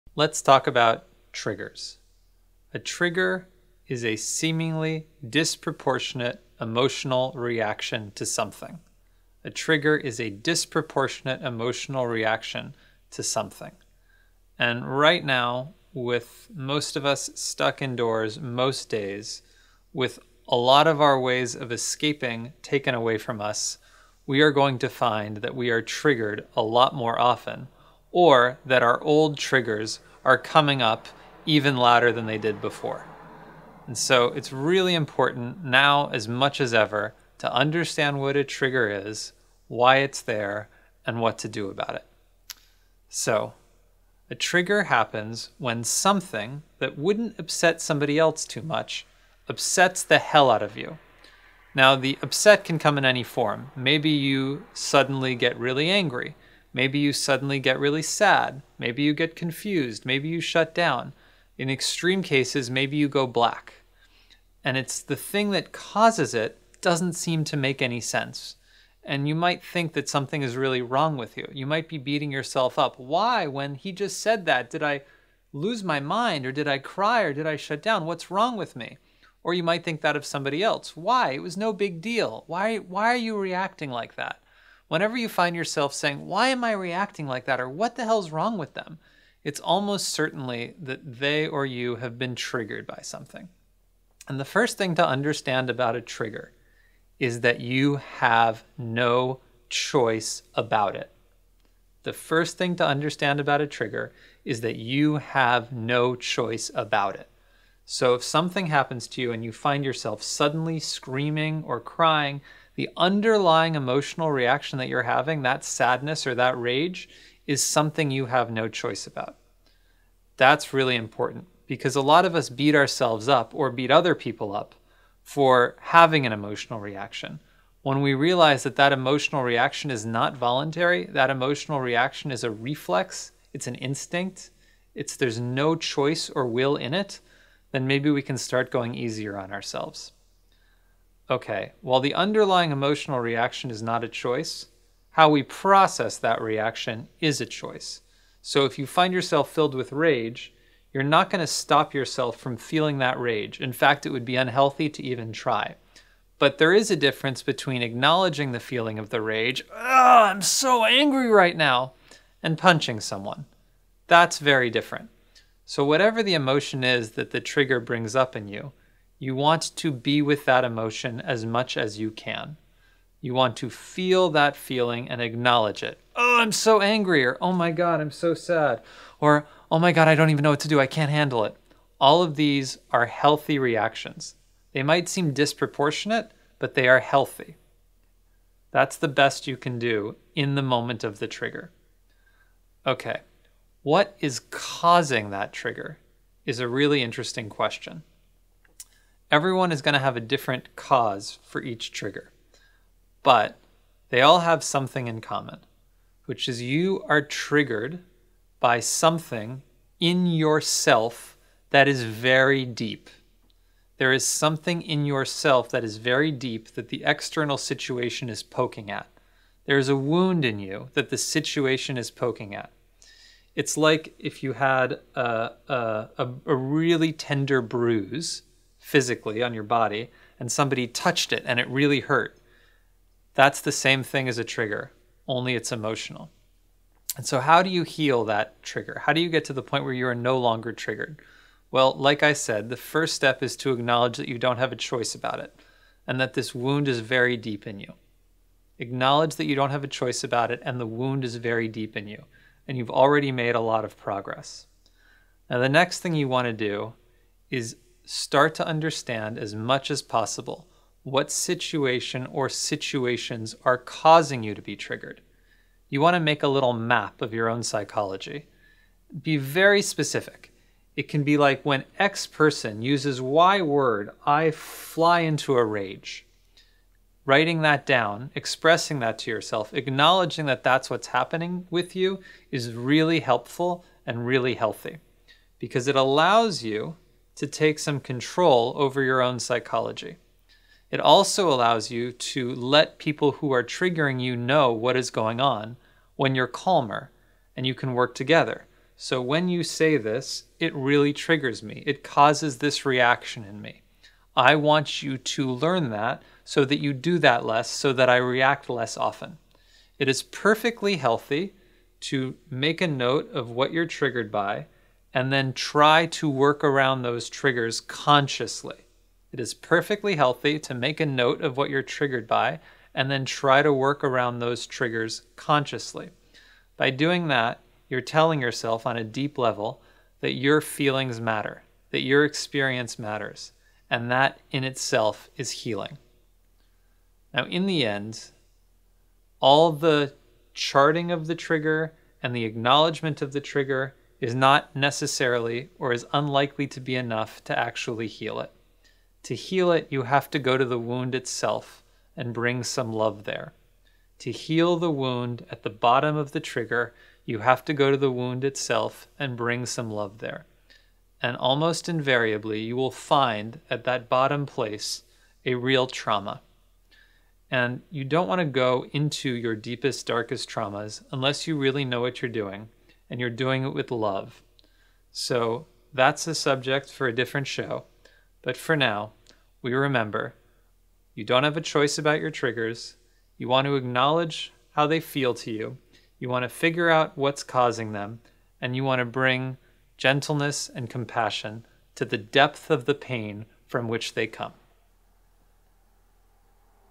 Let's Talk About Triggers
This video explains what a trigger is, what to do about it, and where triggers come from. Just by watching it, you'll end up feeling more compassionate toward yourself and others.